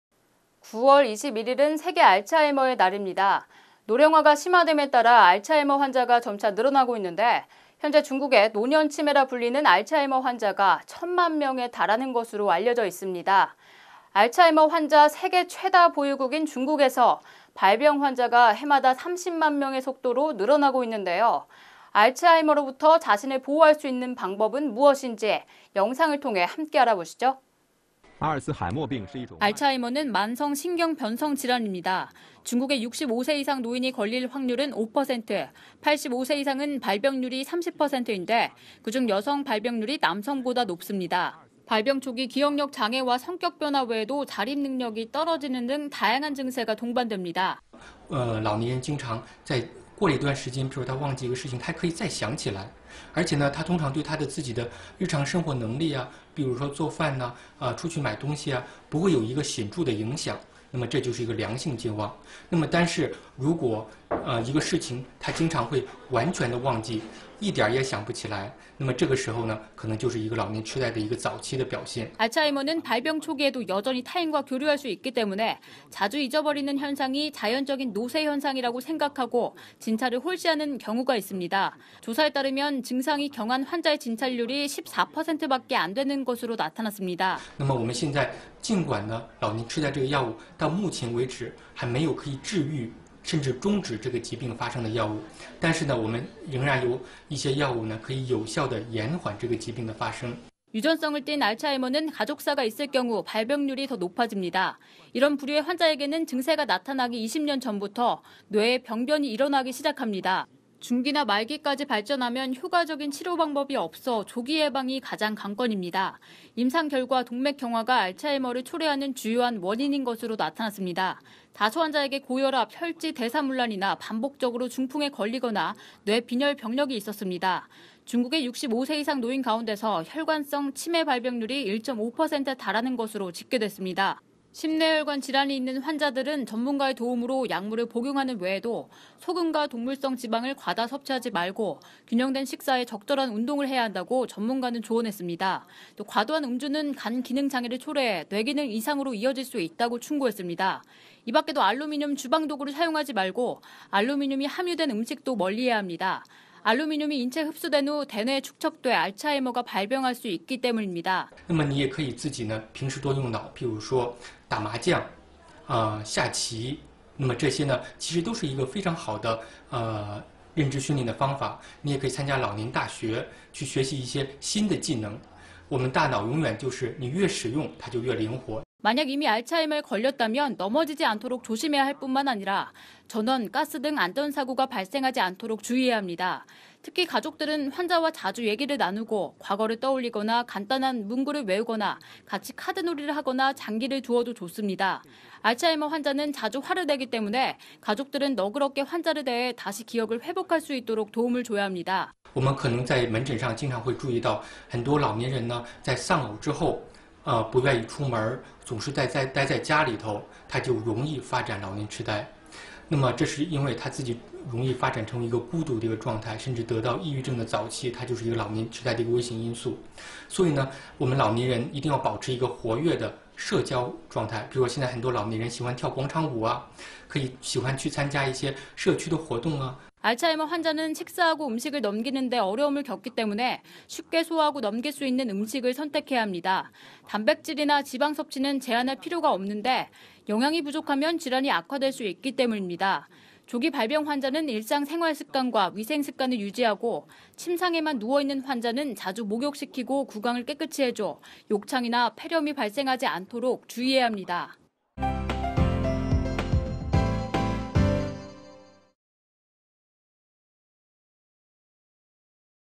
중국, 노령화 심화로 알츠하이머 발병 증가세
9월 21일은 세계 알츠하이머의 날입니다. 노령화가 심화됨에 따라 알츠하이머 환자가 점차 늘어나고 있는데 현재 중국에 노년치매라 불리는 알츠하이머 환자가 천 만 명에 달하는 것으로 알려져 있습니다. 알츠하이머 환자 세계 최다 보유국인 중국에서 발병환자가 해마다 30만 명의 속도로 늘어나고 있는데요. 알츠하이머로부터 자신을 보호할 수 있는 방법은 무엇인지 영상을 통해 함께 알아보시죠.